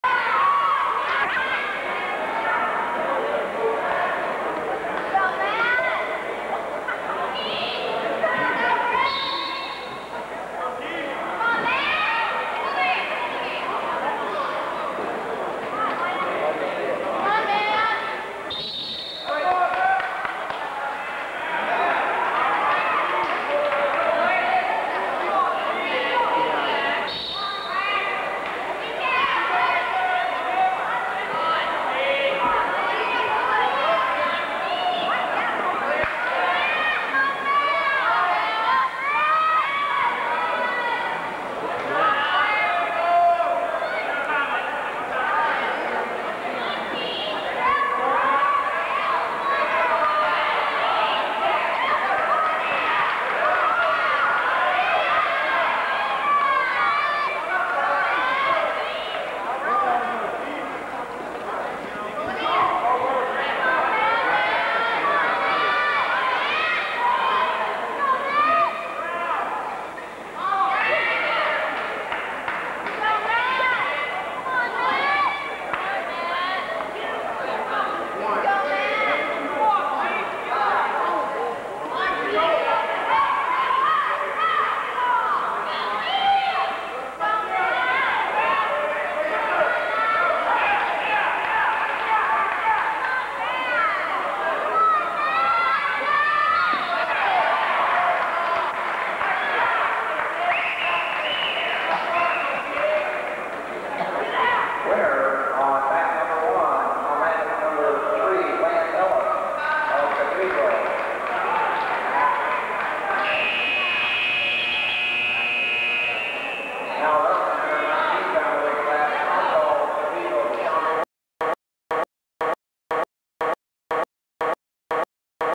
IHSAA 1987 1988 Muncie South Invitation 112 lbs Lance Ellis (Cathedral) vs Matt Love (Western)
IHSAA Wrestling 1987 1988 Muncie South Invitation 112 lbs Lance Ellis (Cathedral) vs Matt Love (Western).  Round 3 - Final.